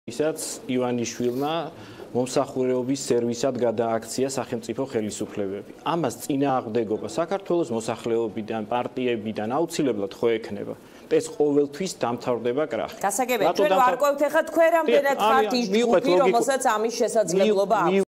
შოთა მალაშხია დიანა ტრაპაიძის დღის ამბებში
ივანიშვილმა მომსახურეობის სერვისად გადააქცია სახელმწიფო ხელისუფლება - შოთა მალაშხია